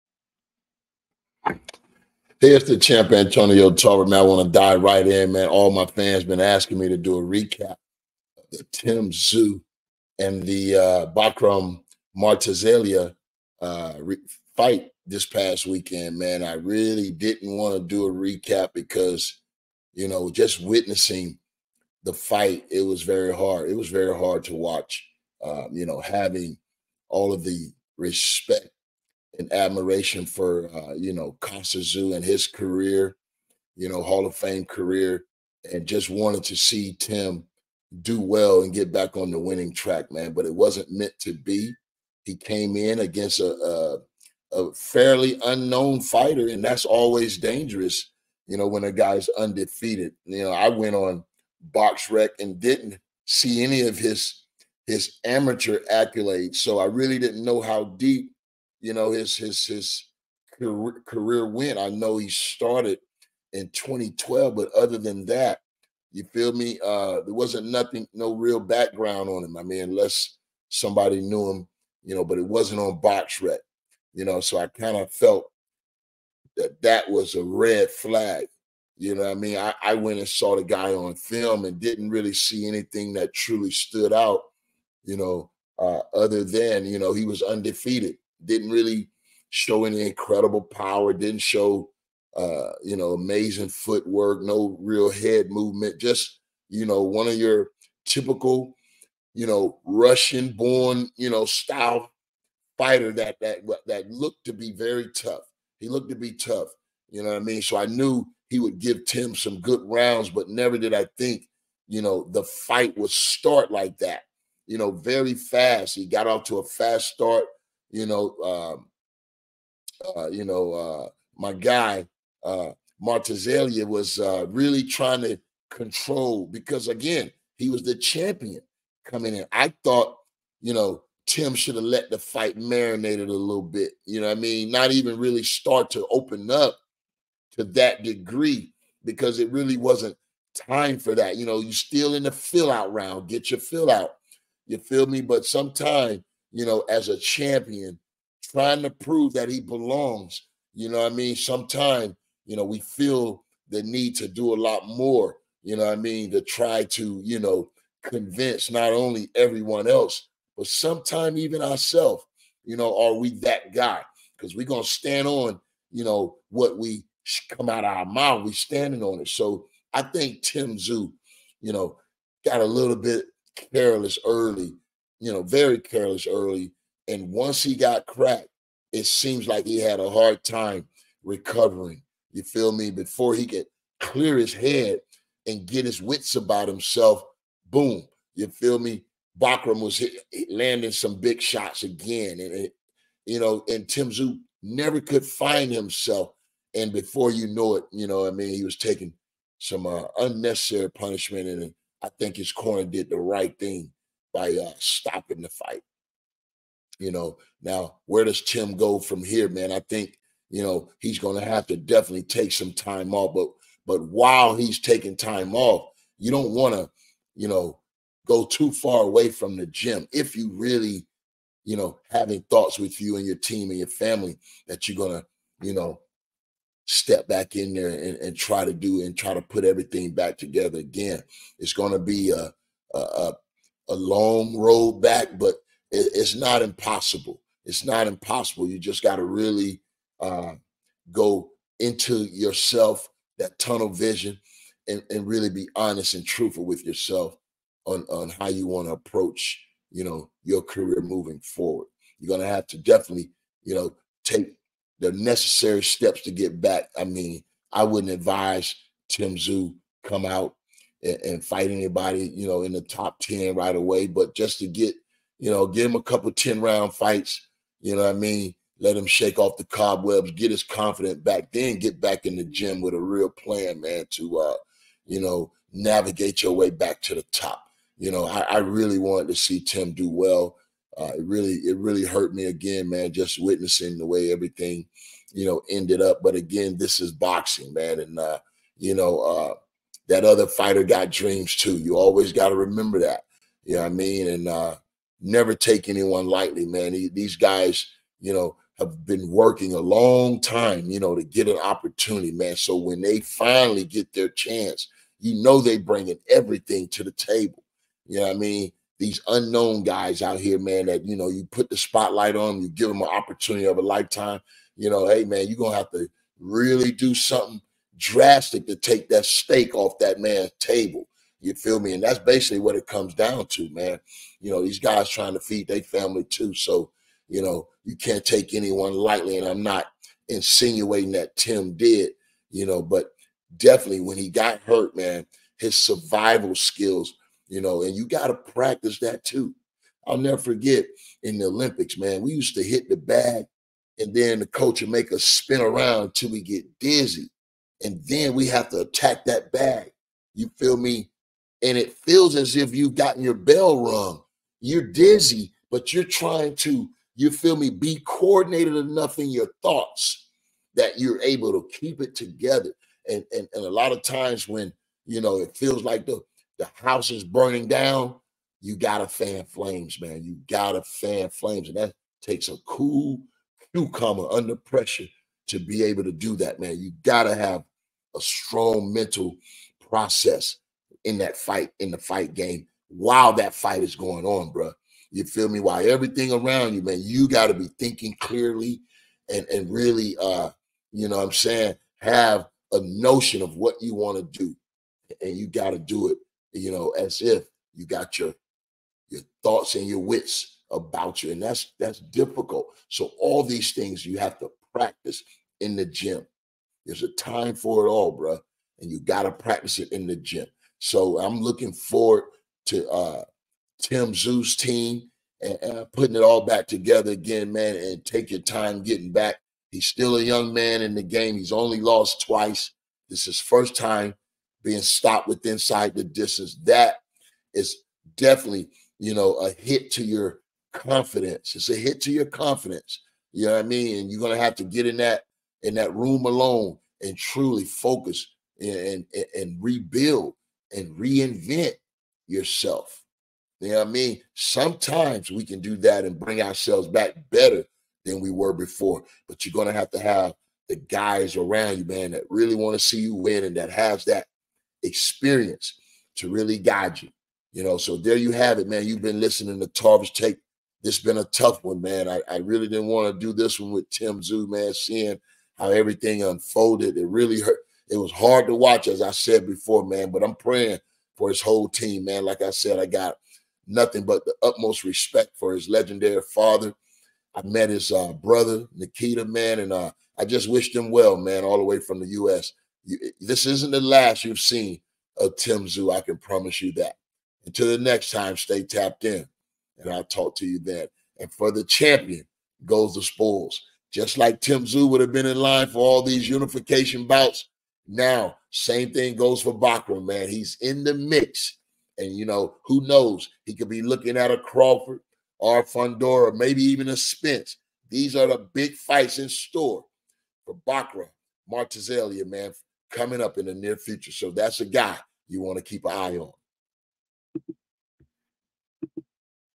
TarversTake The Re-Cap Tim Tszyu vs Bakhram Murtazaliev
Bakhram Murtazaliev retains his IBF Jr. Middleweight Title in a Shocker In Florida by KO Tim Tszyu in what was expected to be a successful comeback for the former champion. After some early exchanges in the 1st round Murtazaliev landed a short devastating hook that dropped Tszyu and after surviving the 1st round he was never able to turn the tide after being hurt again in the third with another crushing hook Bakhram continued to apply pressure until referee Chris Young was forced to step in as Tim Tszyu corner was throwing in the towel to halt the bout at 1:05 of the third round.